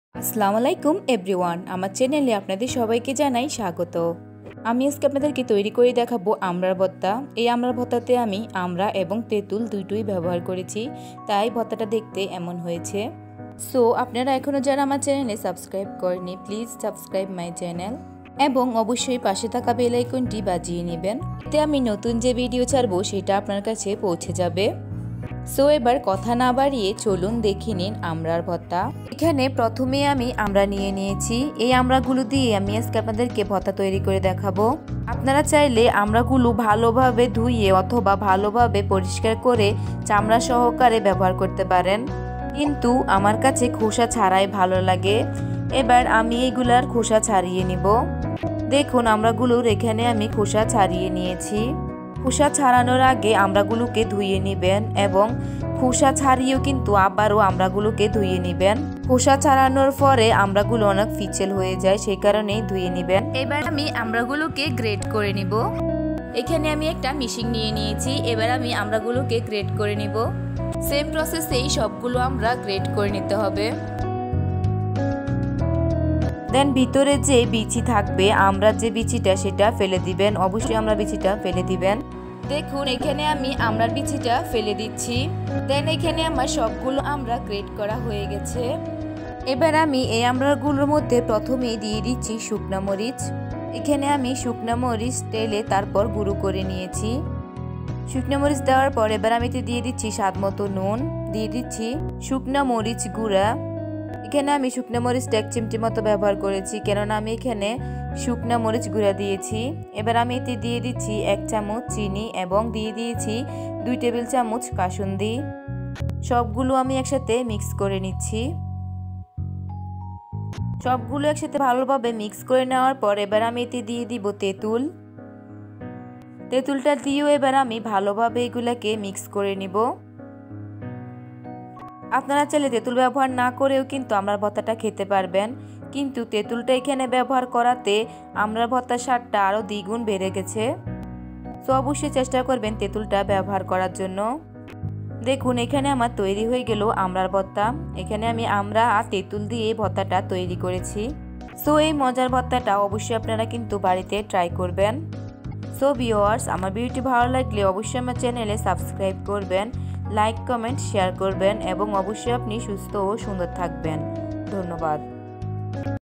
★আমড়া, তেঁতুল ভর্তা রেসিপি ||ভিটামিন 'সি' সমৃদ্ধ আমড়া ও তেঁতুলের ভর্তা
★ আমড়া,তেঁতুল ভর্তা রেসিপি ||
উপকরণঃ-
1.আমড়া
2.তেঁতুল
3.চিনি
4.নুন
5.গুঁড়া মরিচ
6.কাসুন্দি
7.শুকনা মরিচ